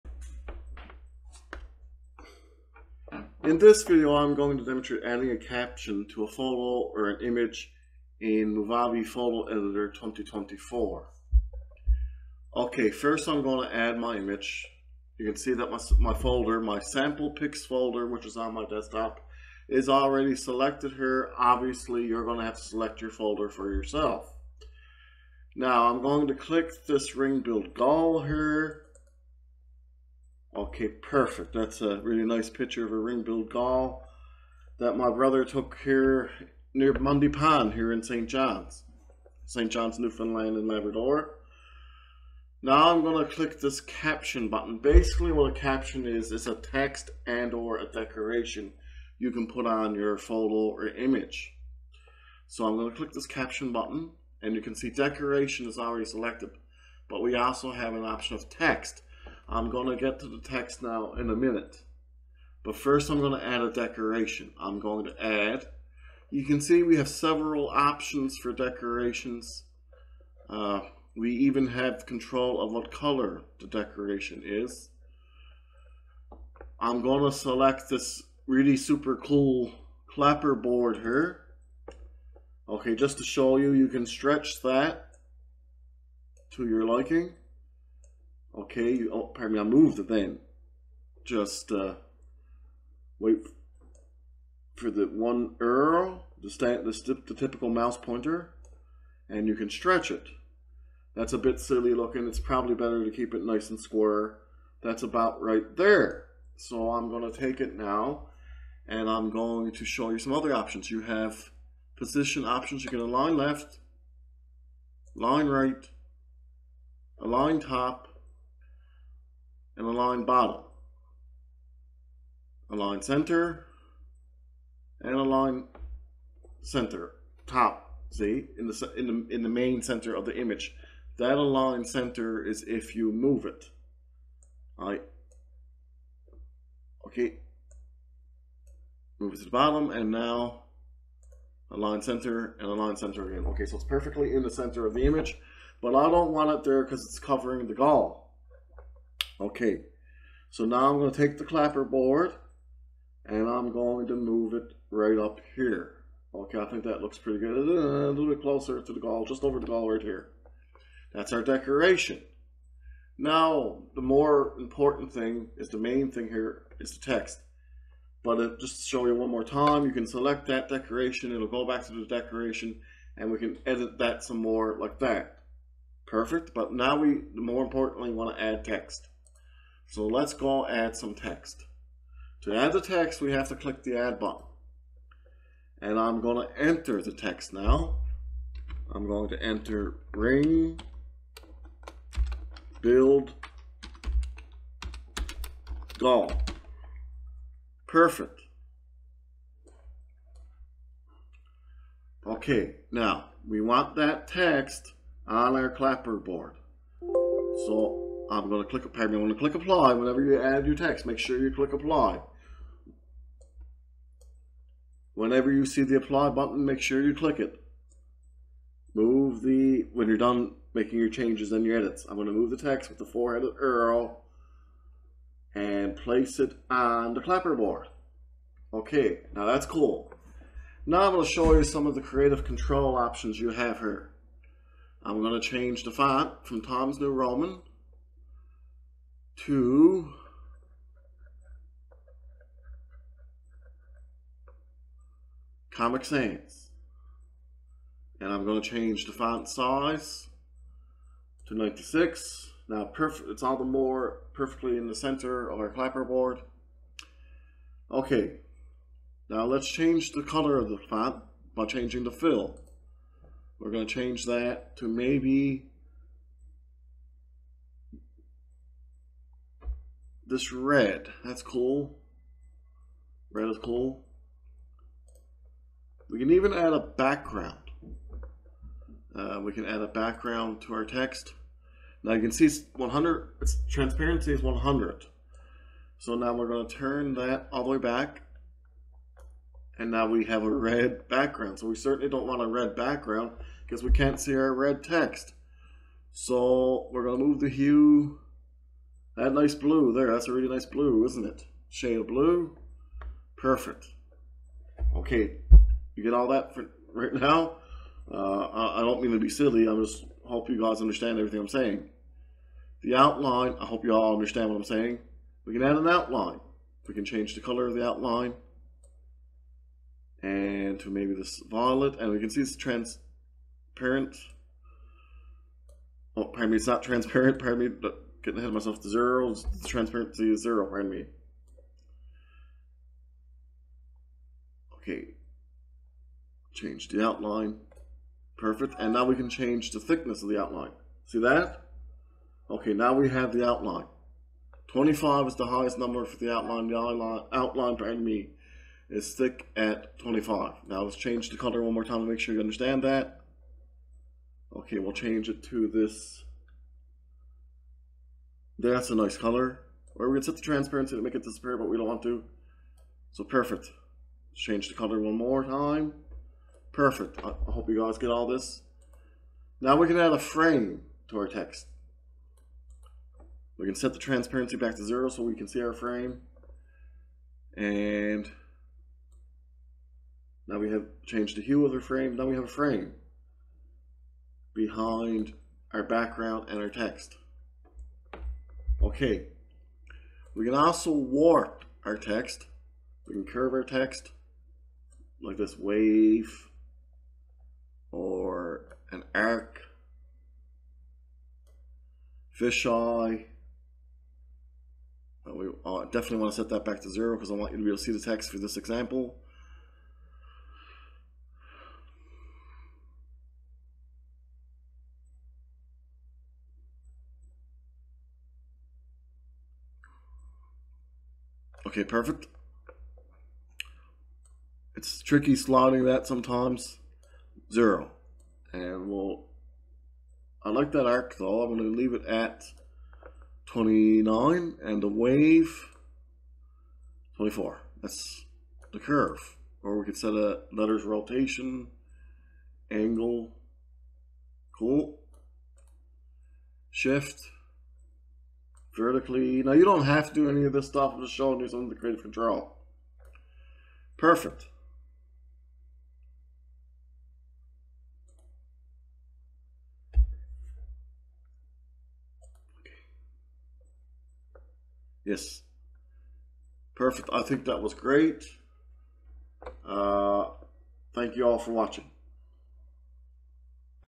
Add A Caption In Movavi Photo Editor 2024
In this video we demonstrate adding a caption in Movavi Photo Editor 2024.